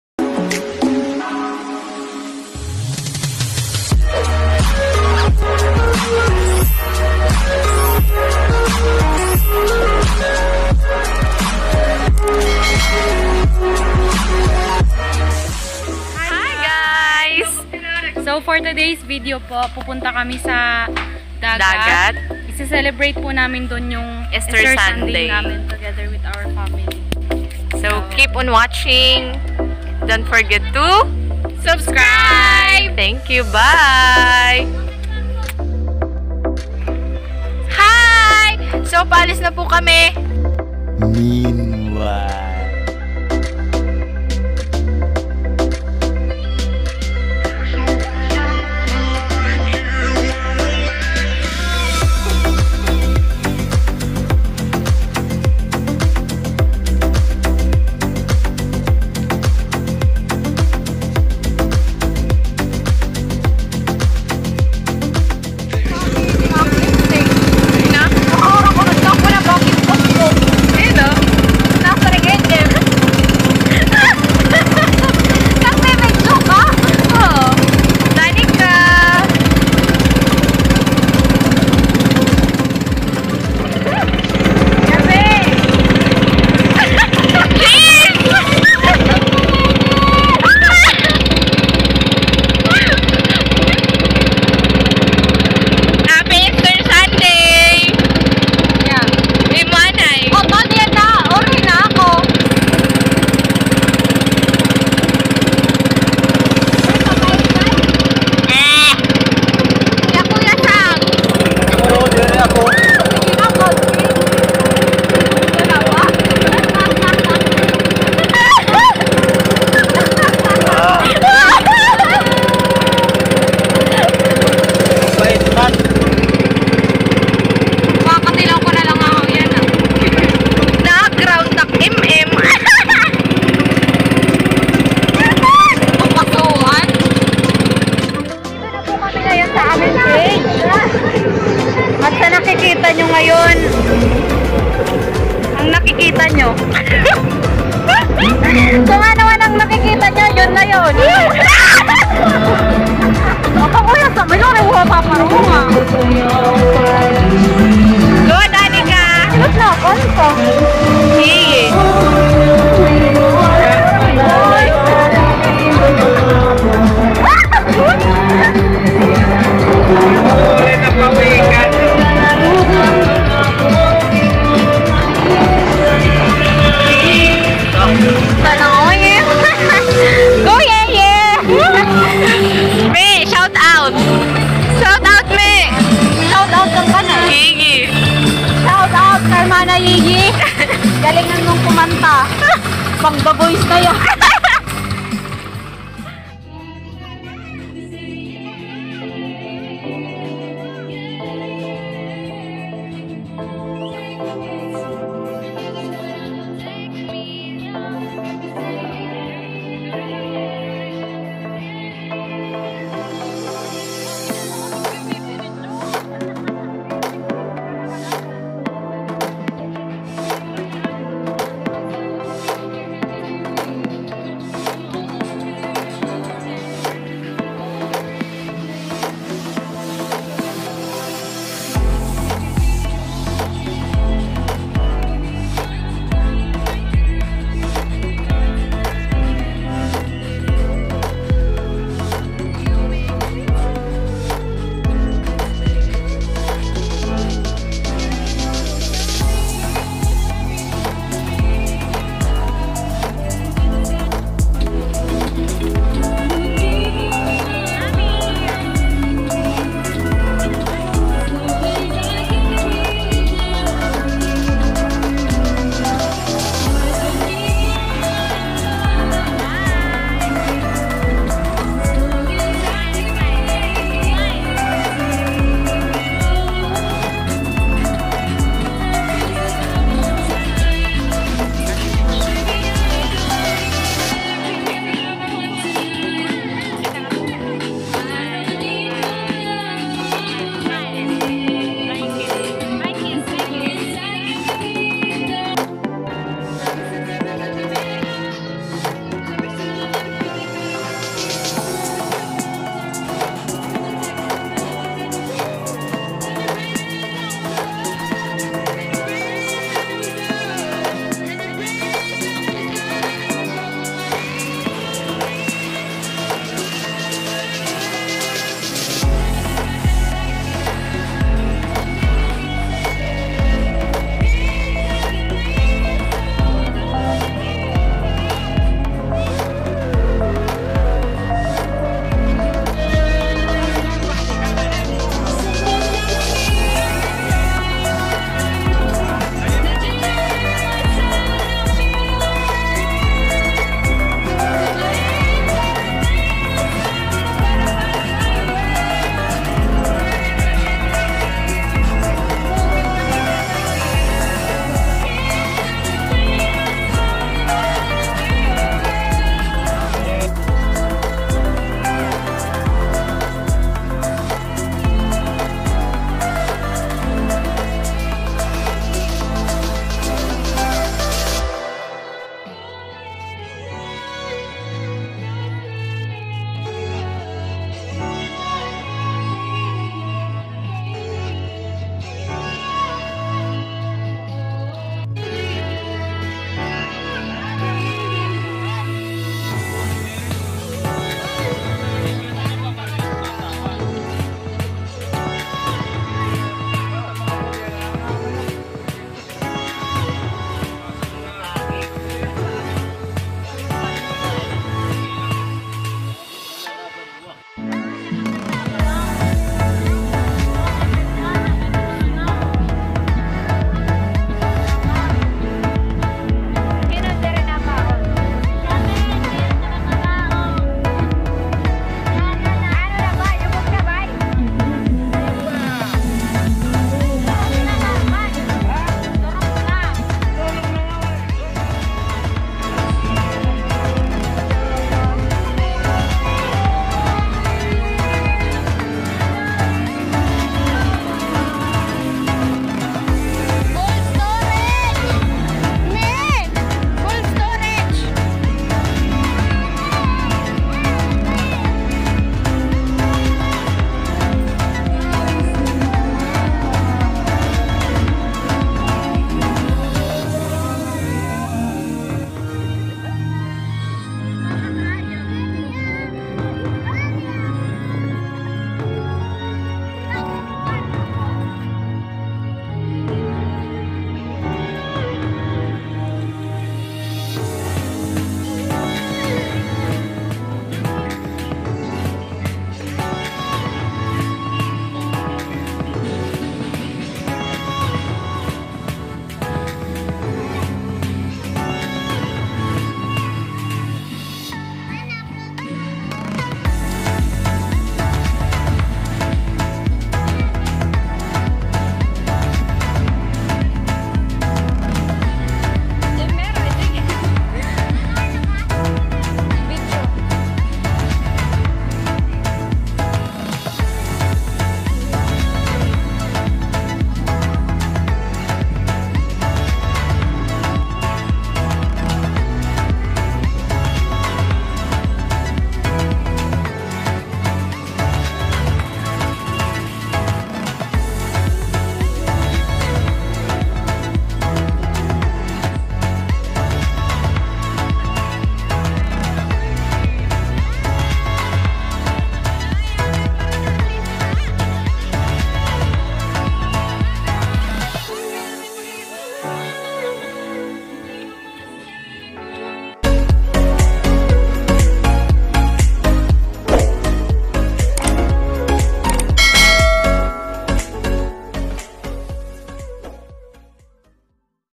EASTER SUNDAY 2021 | FAMBAM | DAGAT | BUHAY PROBINSYA | VLOG 9
Helloooooo!
Happy Easter Sunday Everyone!😇

Just want to share our Celebration for Easter Sunday☺️

Godbless and Stay safe!💕